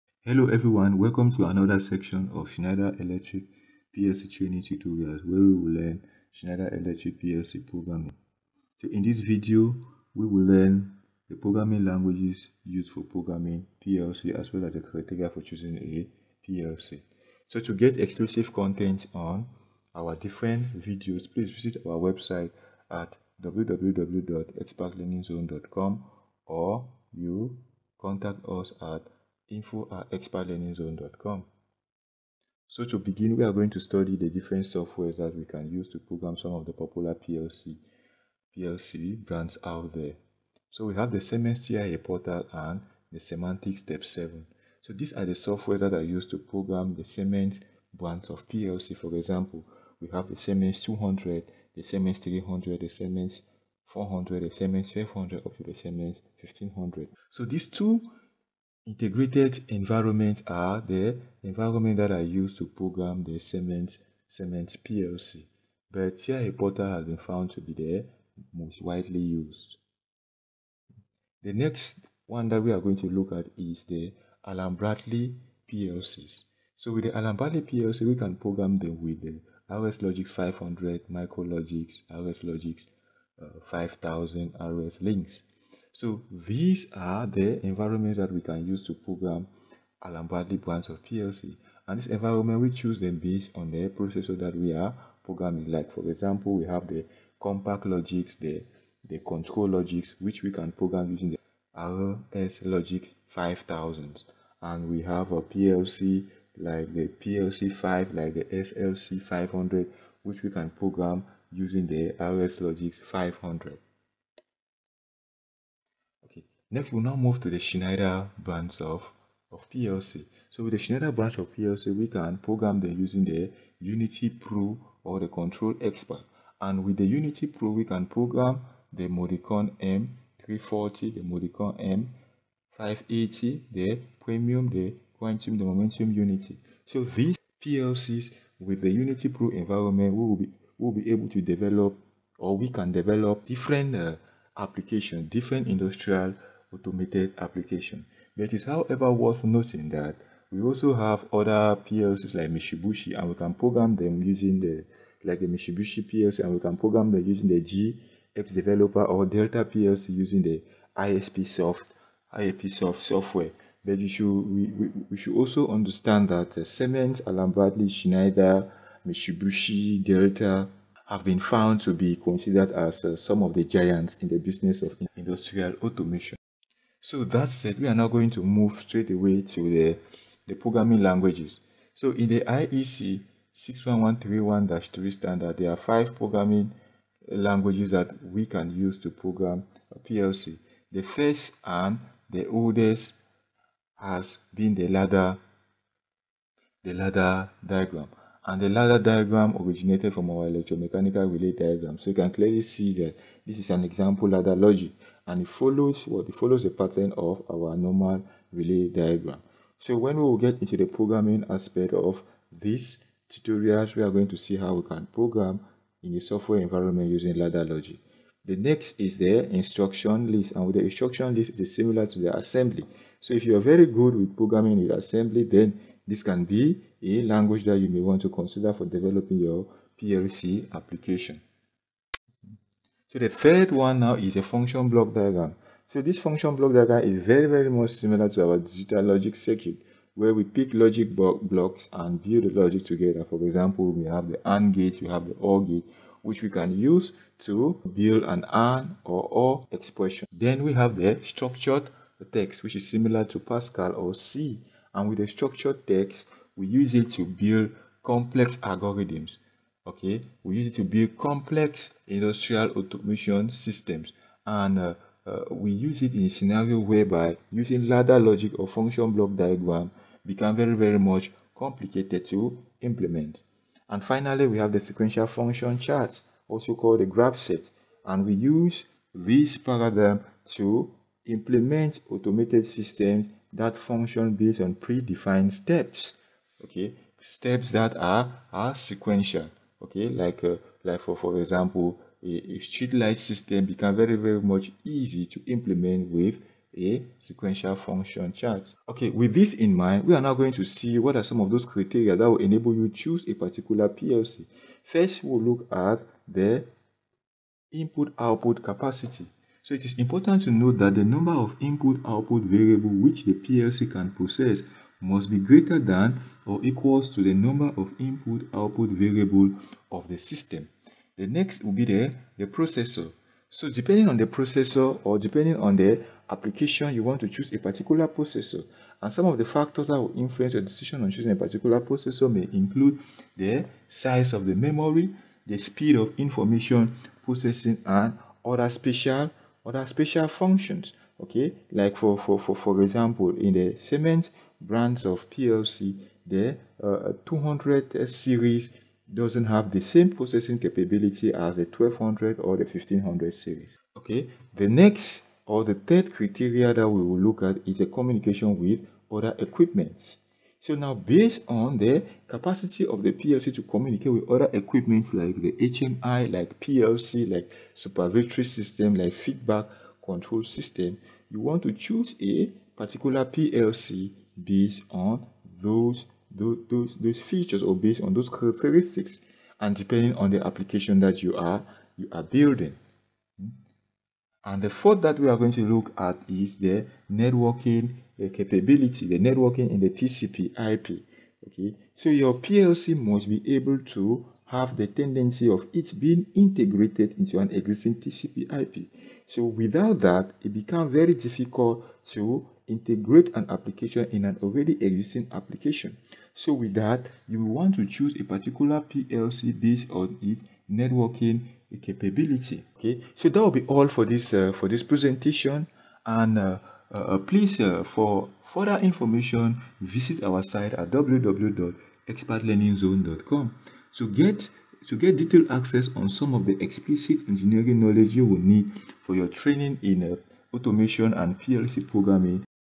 L2: Unity Pro (Control Expert) Schneider PLC - PLC Software Programming Languages
Hello everyone, welcome to another section of Schneider Electric PLC Training Tutorials where you will learn Schneider Electric PLC programming (unity pro). In this video we will learn the programming languages used for PLC programming and the criteria for choosing a PLC

0:00 SCHNEIDER ELECTRIC PLC TRAINING TUTORIALS
0:33 Software used for PLC Programming
2:57 PLC Programming Languages
4:59 Criteria for choosing a PLC The choice of a PLC is based on the following criteria.